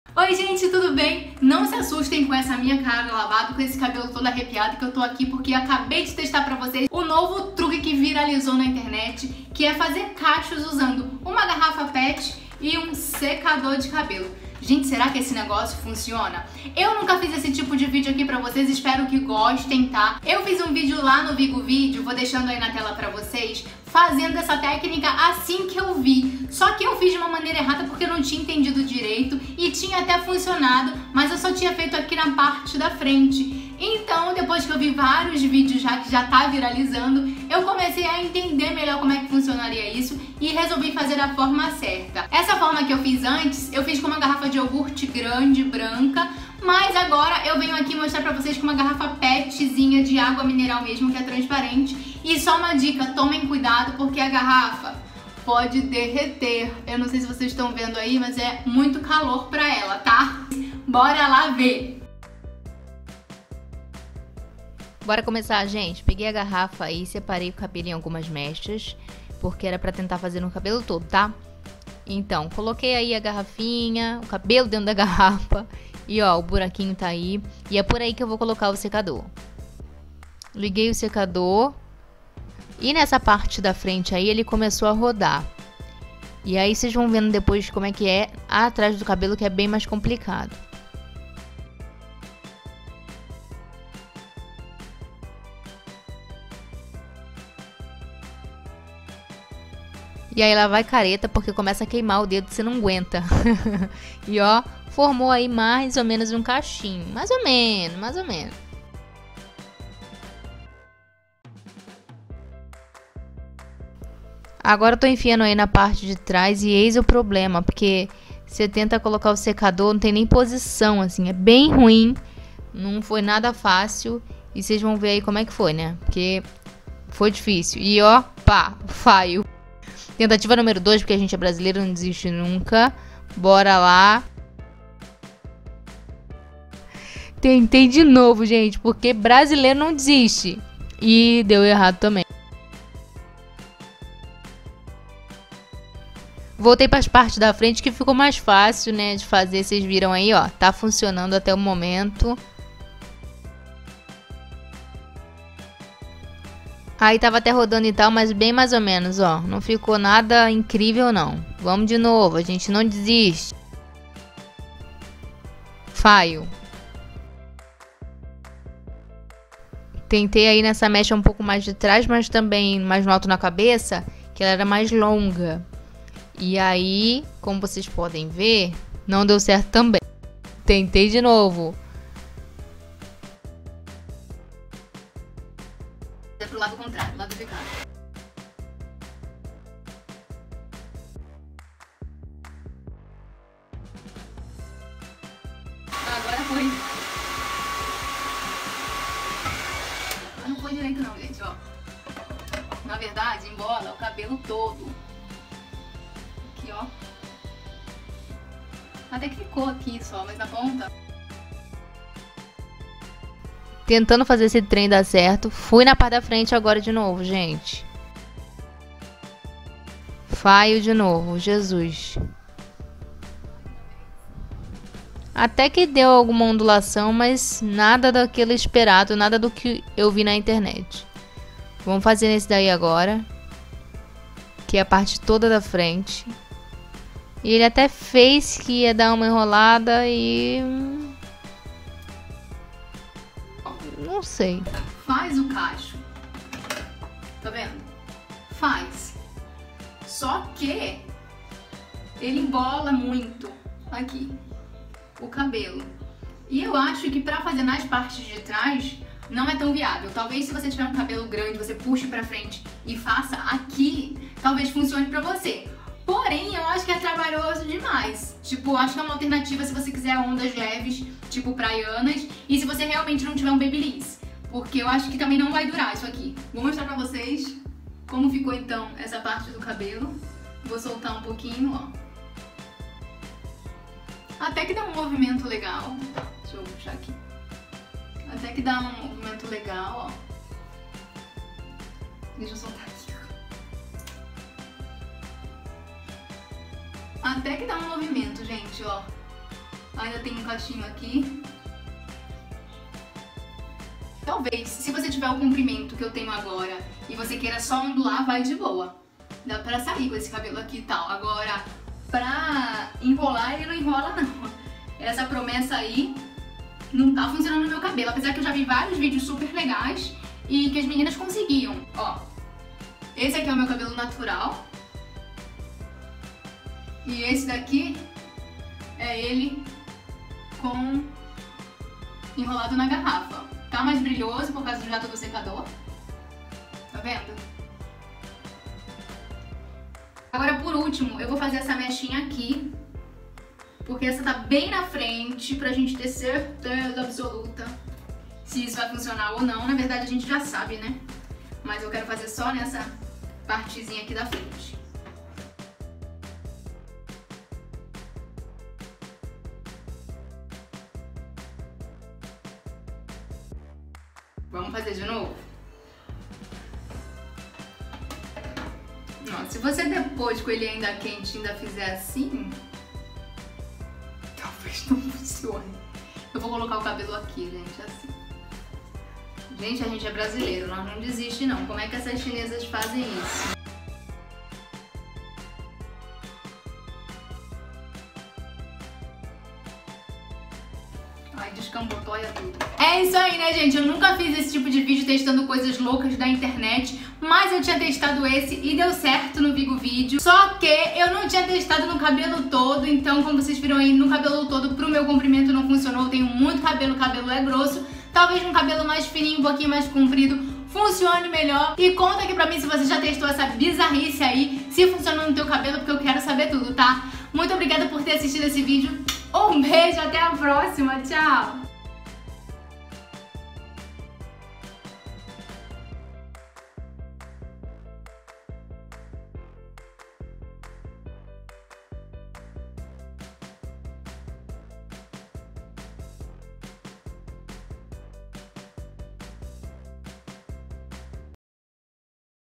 Testando Garrafa Pet para Ondular o Cabelo
Eita truque bizarro da internet, né gente!
Nunca trouxe um vídeo como esse pra vocês, mas hoje resolvi vir aqui e mostrar se realmente funciona o truque de ondular o cabelo usando uma garrafa pet e um secador.
Já vi dando certo com muita gente, será que comigo deu?
Testei em todo o cabelo, vem ver o resultado!

Passa lá no nosso insta novo também: @diypravc

Vigo Vídeo: Paty Básica